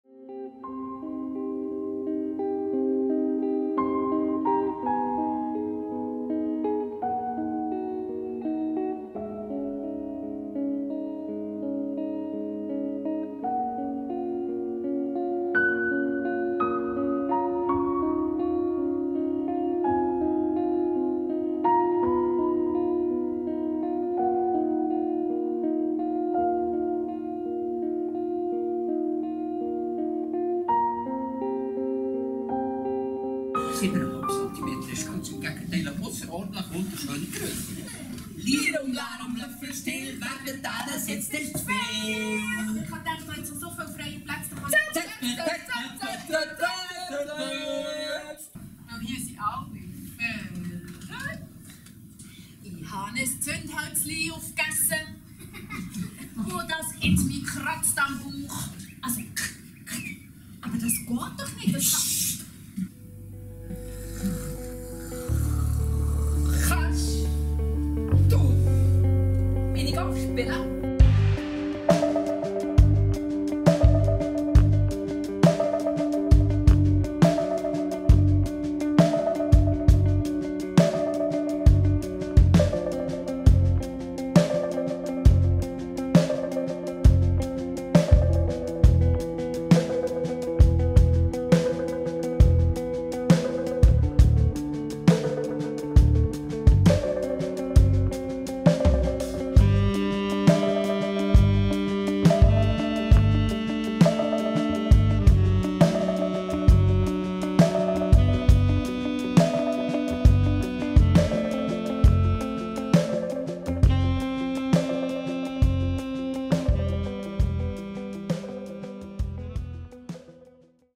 Alice im Wunderland - Trailer Theater Szene
Saison 2024/25, Theater Szene, Bern
Nach der Erzählung von Lewis Carroll in einer Fassung von Mägie Kaspar.
Mit Musik des Akku Quintets, Manuel Pasquinelli, Illustrationen von Jacky Gleich, Kostüme von Salome Egger
Mit: Fabienne Trüssel (Alice), Daniel Rothenbühler (Vater, Pfau, Raupe, Hutmacher, Diener), Laura Locher (Weisses Kaninchen, Maus, Grinsekatze, Schnapphase, Königin/König)
Regie: Mägie Kaspar, Oeil Extérieur: Miriam Jenni